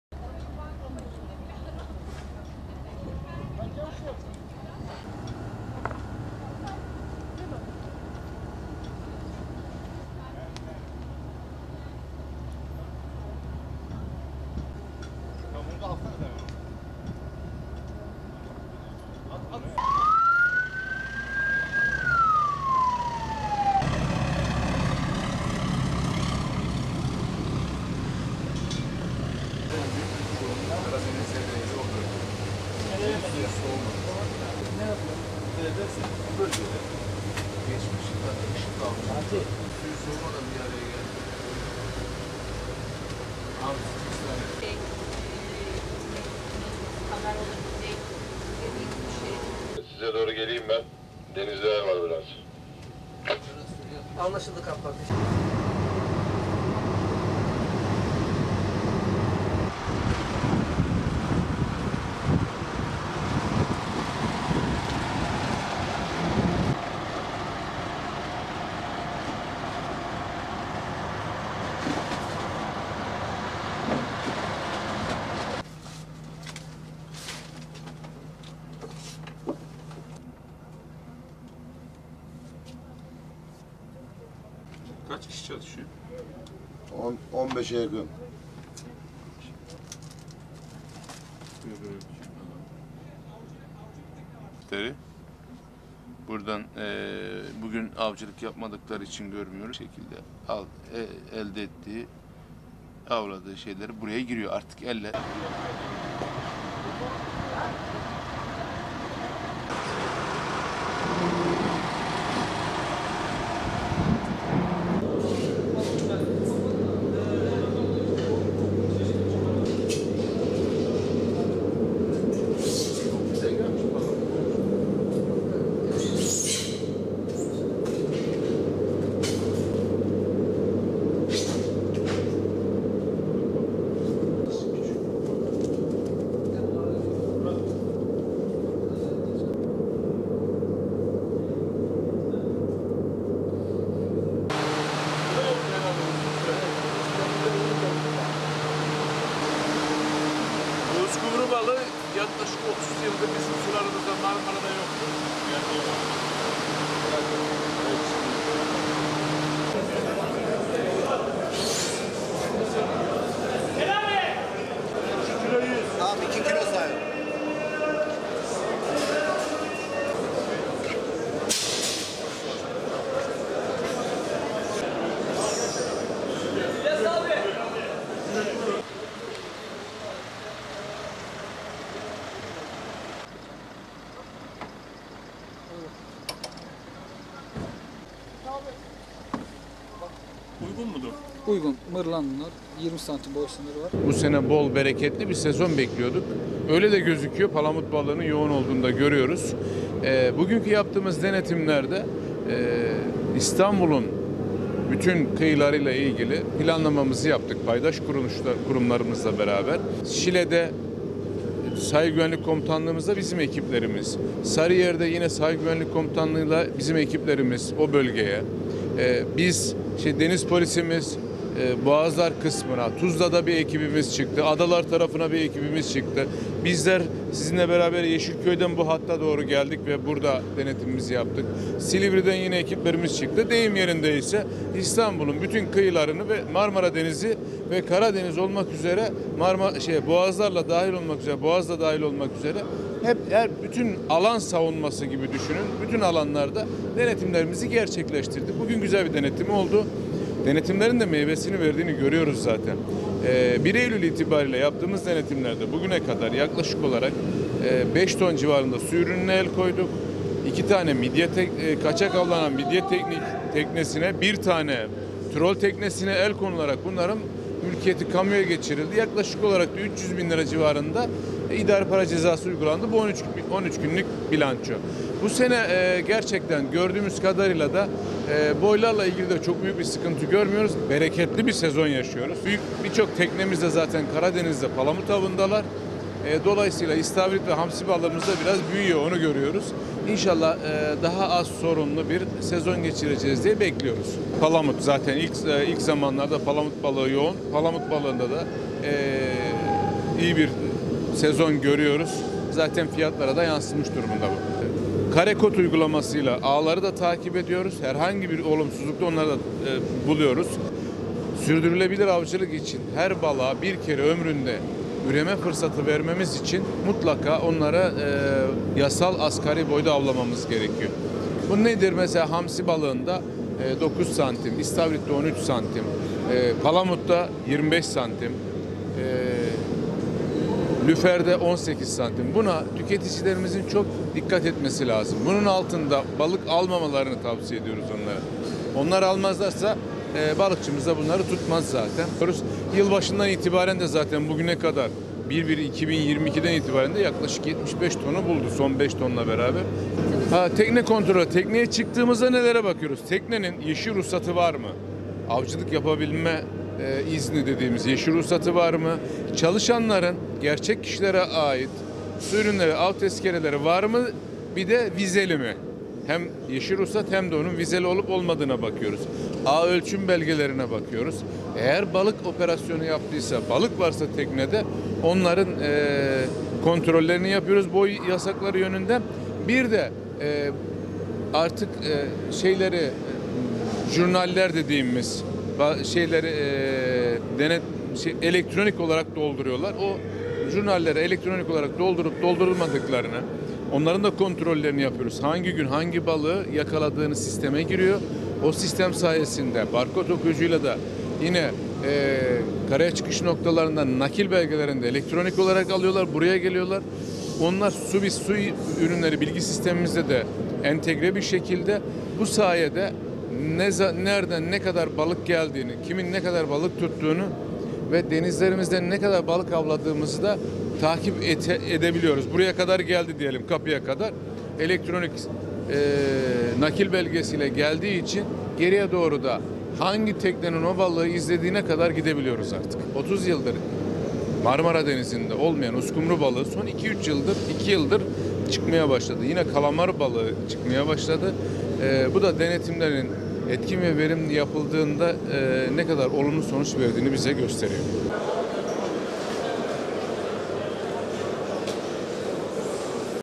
Denizlerde palamut bolluğu yaşanıyor
Av sezonunun açılmasıyla İstanbul'da su ürünlerine yönelik denetimler de sıklaştı. İstanbul İl Tarım ve Orman Müdürlüğü ve Sahil Güvenlik ekipleri Karadeniz ve Gürpınar Su Ürünleri Hali'nde denetim yaptı.